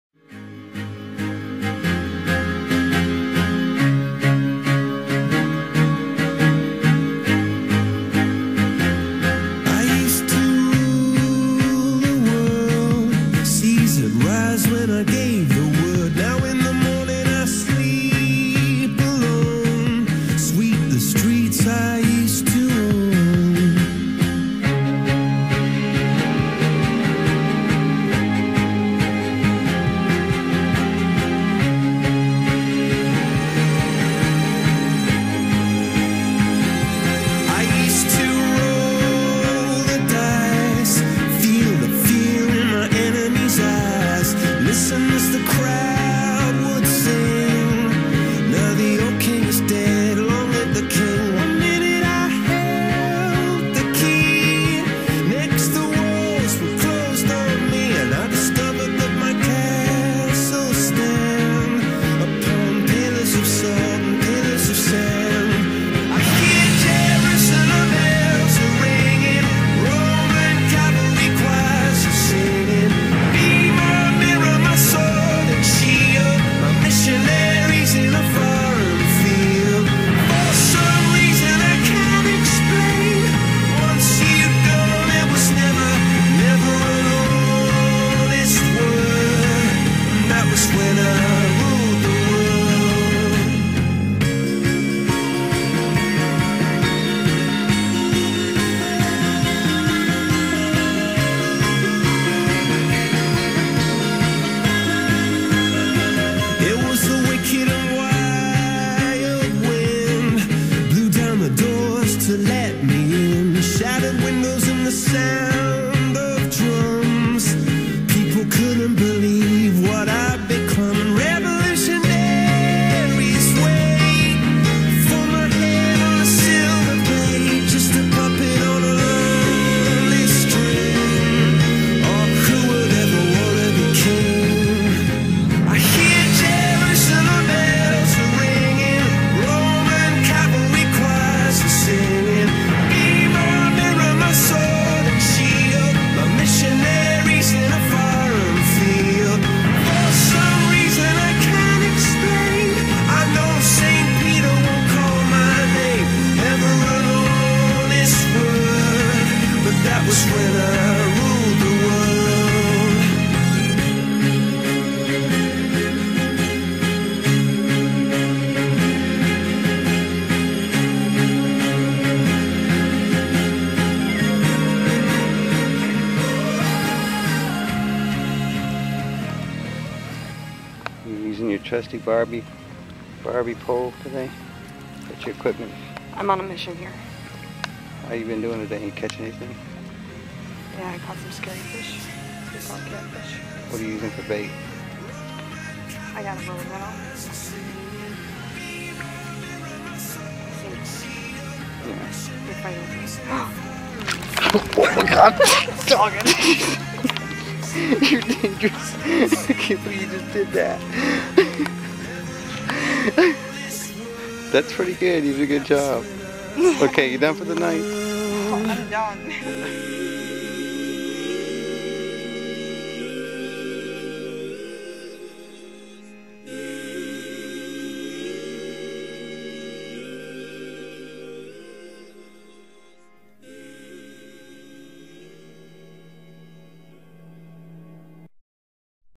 North to South
My 2 weeks traveling from Wisconsin to Texas and everything in between.  Including but not limited to Proffessional foosball, Gator hunting and Monuments..to being fish slapped in Galveston Bay. Surprise ending!